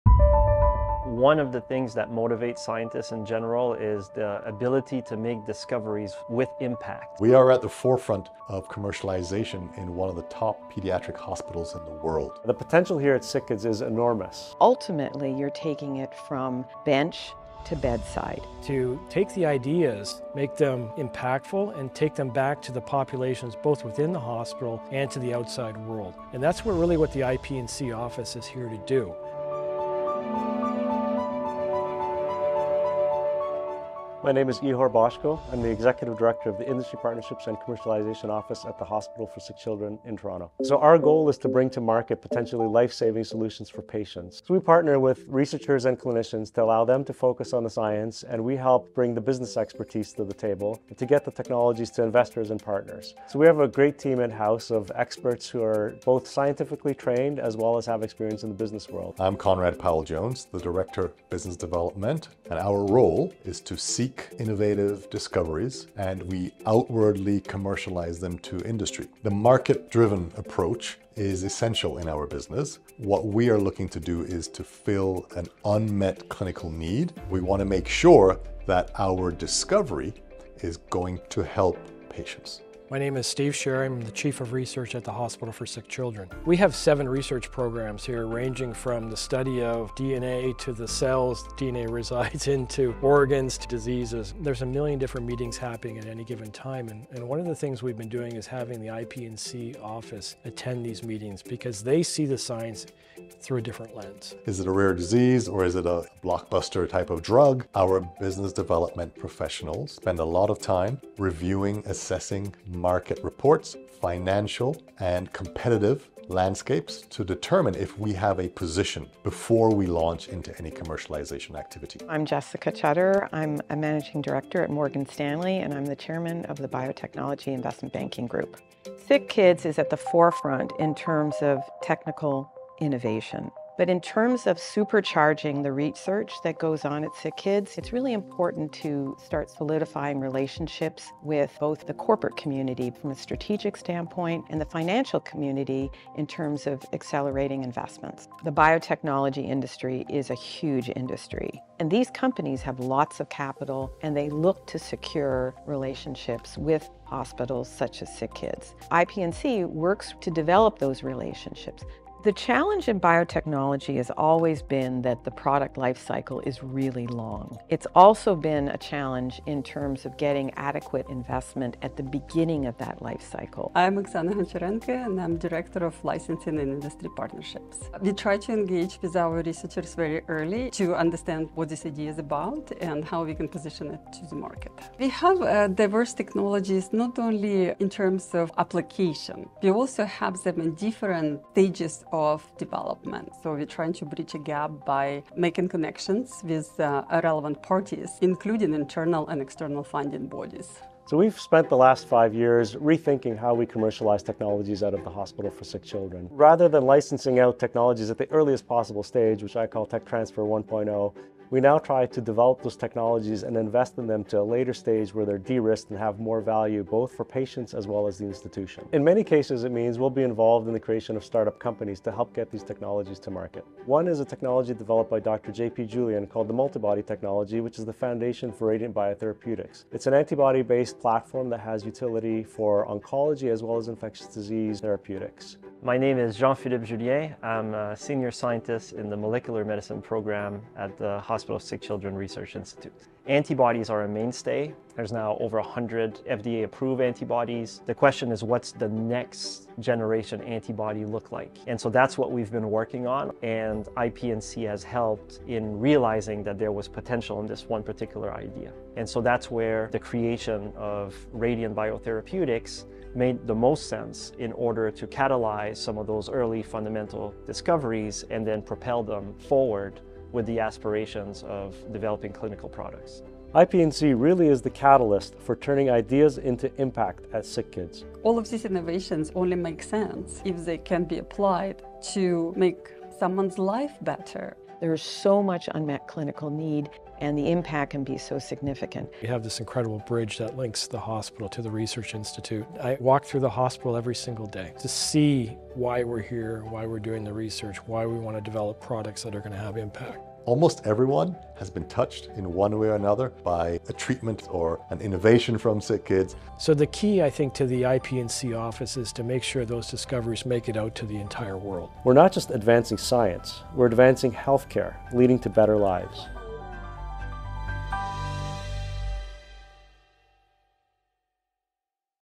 Bringing SickKids innovations to market | Industry Partnerships & Commercialization
Industry Partnerships & Commercialization (IP&C) at The Hospital for Sick Children (SickKids) is a catalyst for innovation in paediatric health care. Through strategic partnerships with industry leaders, academia and the broader health ecosystem, IP&C facilitates the translation of ground-breaking research into tangible solutions that can improve the lives of patients worldwide.

To discover the pivotal role IP&C plays at SickKids in advancing innovation